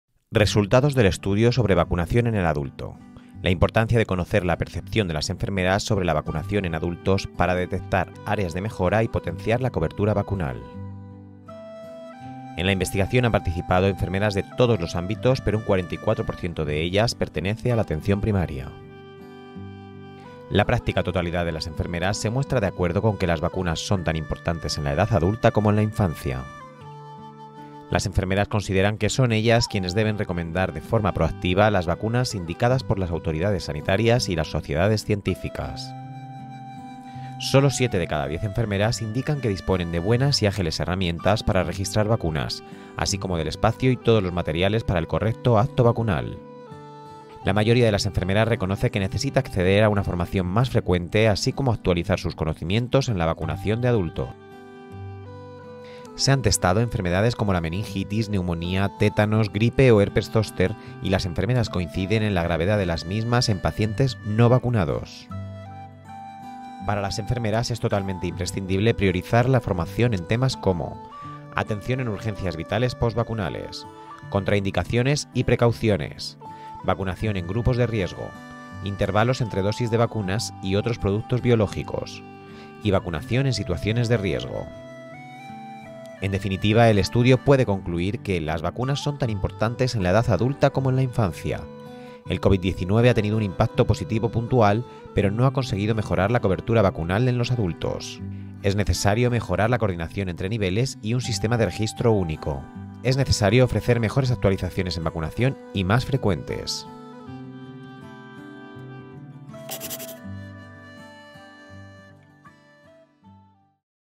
Estudio sobre vacunación del adulto
El IV Congreso Nacional de Enfermería y Vacunas ha acogido la presentación  un vídeo animado que ha presentado los principales resultados de la encuesta de percepción enfermera de la vacunación del adulto en nuestro país llevada a cabo por el Consejo General de Enfermería y la Asociación Nacional de Enfermería y Vacunas (ANENVAC).